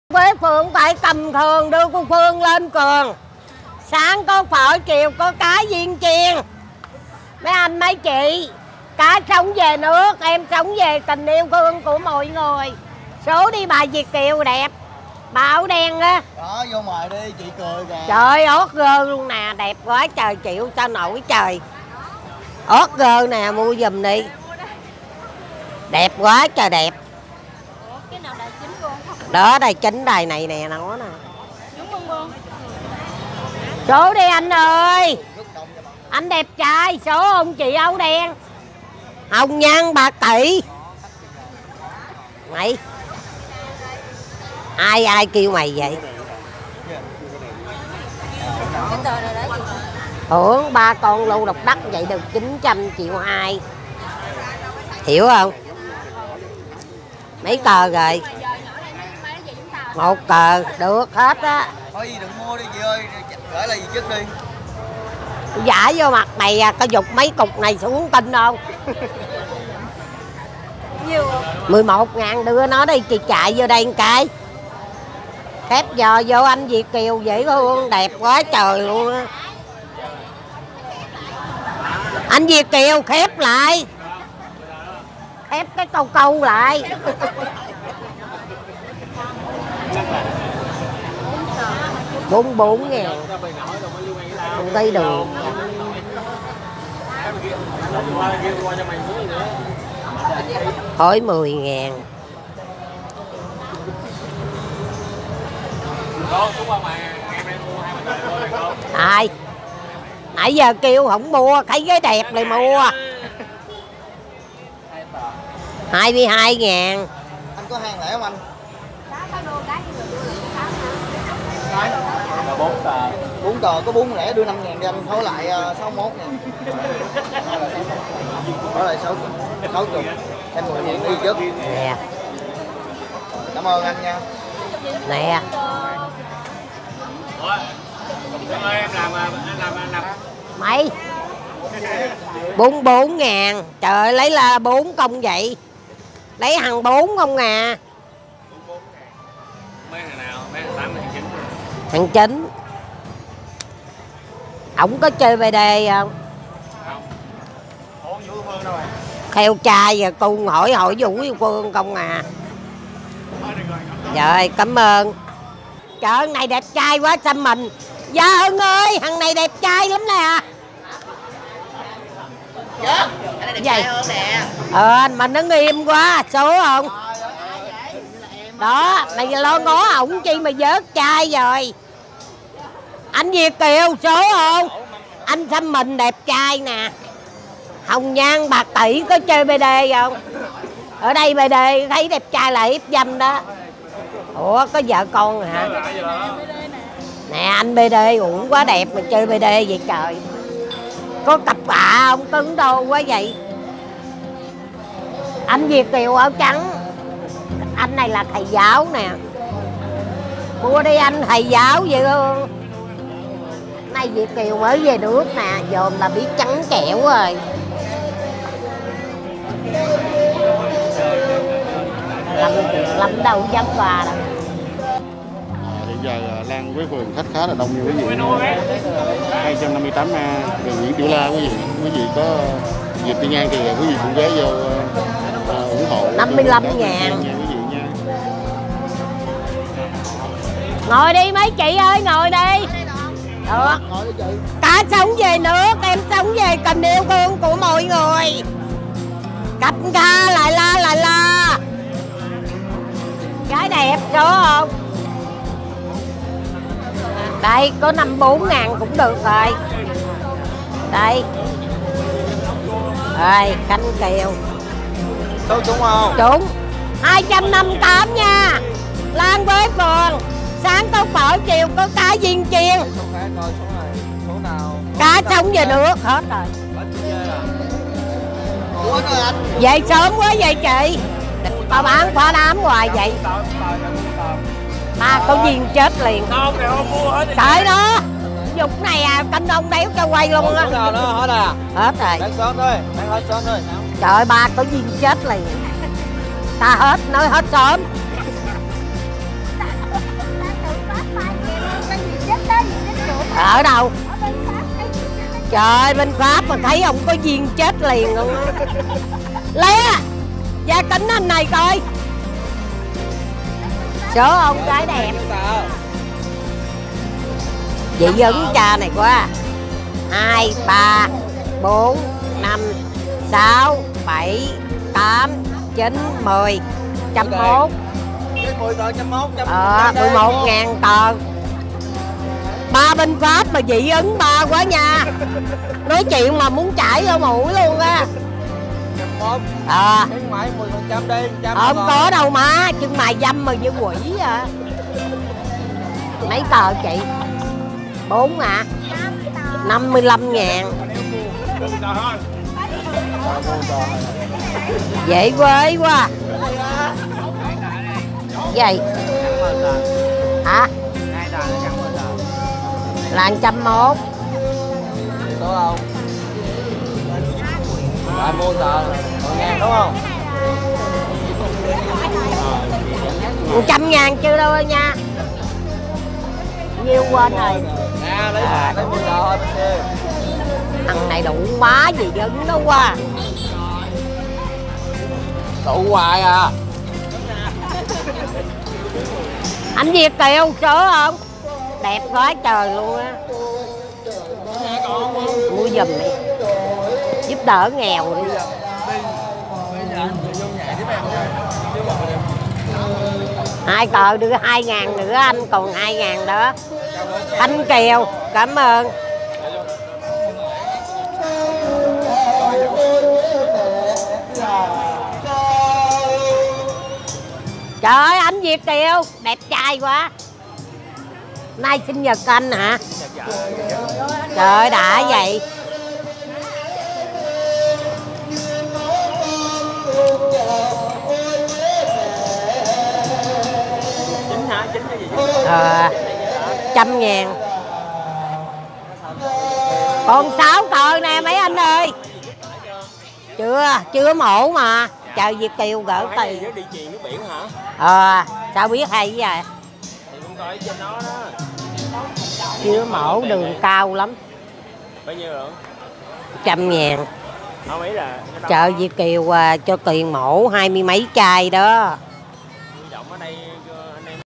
Dì chức kêu khách xếp cái phao câu lại cười đau bụng | Thảo Sài Gòn
Dì chức vừa bán vé số vừa chọc khách hàng cười đau bụng